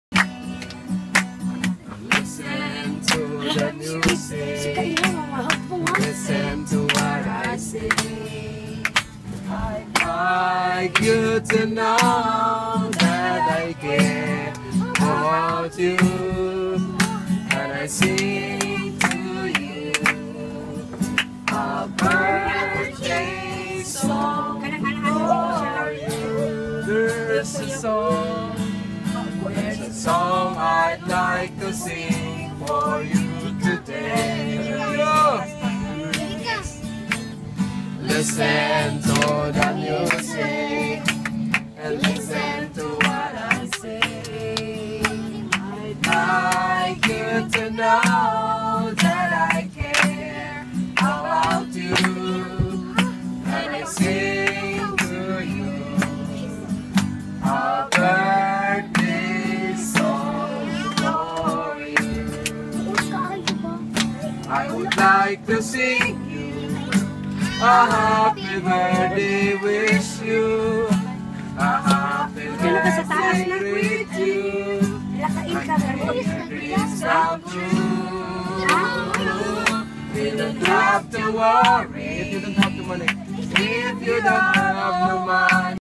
Happy birthday Ate Jean
A joyful celebration of a church friend at their cute house.